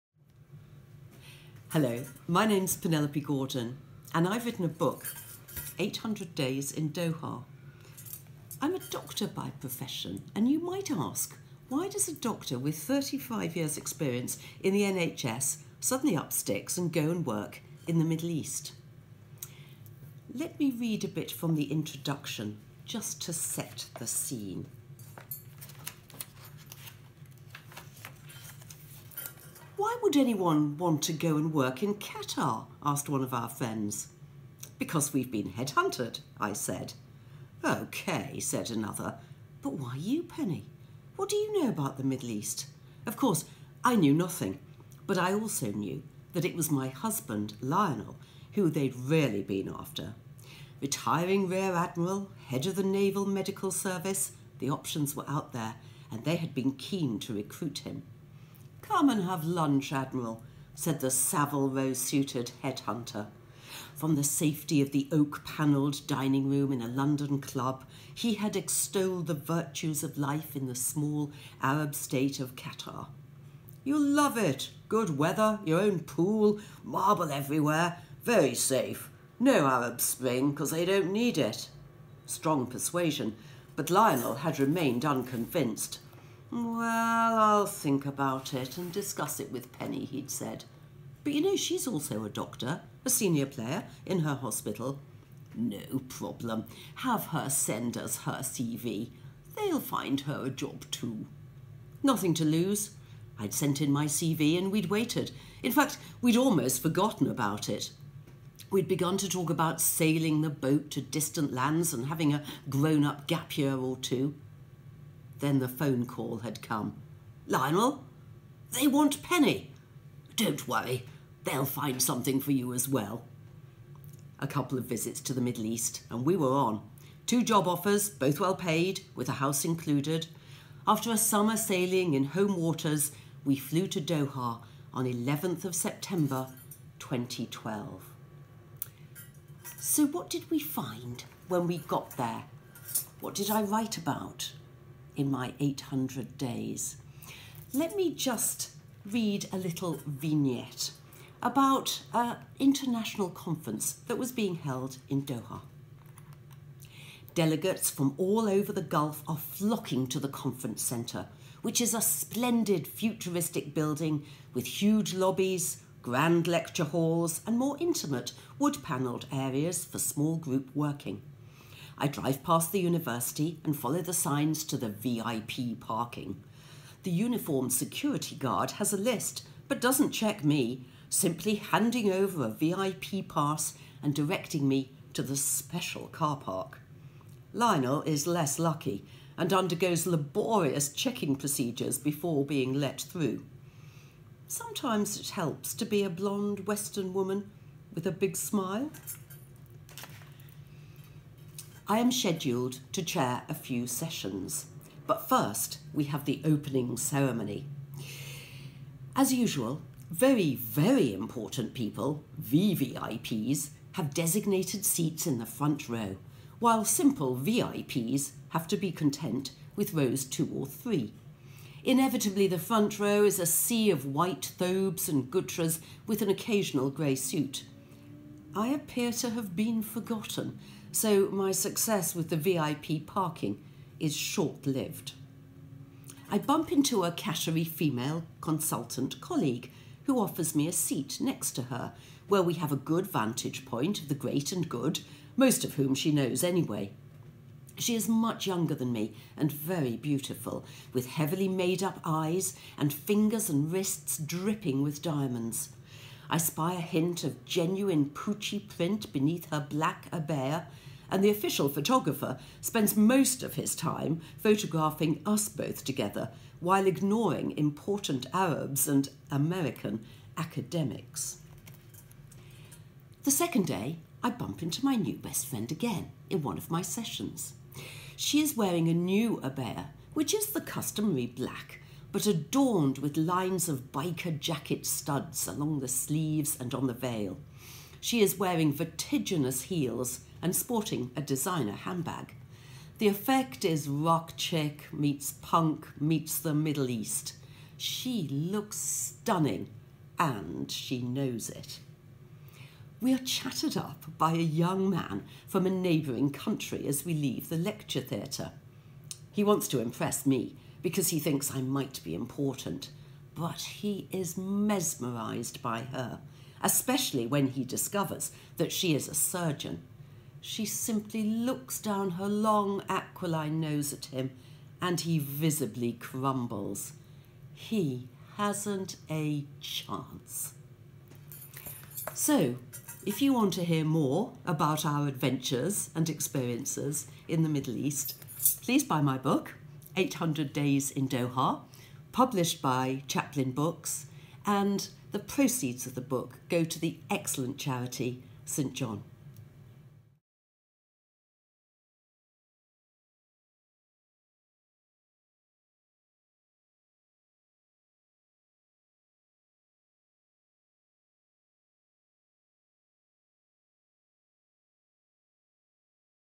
Penelope Gordon reads an extract from '800 Days in Doha'
When Dr Penelope Gordon was headhunted to go and work in Doha, Qatar, her sole experience of the Middle East had been a brief luxury break in Dubai. In Doha, she headed up medical leadership and education in a set of hospitals ... and found it a very different experience from working in the NHS. In this extract from her book, '800 Days in Doha', she shows how she managed to find the humour in dealing with a culture that could be baffling and opaque, and turn her experience into an adventure.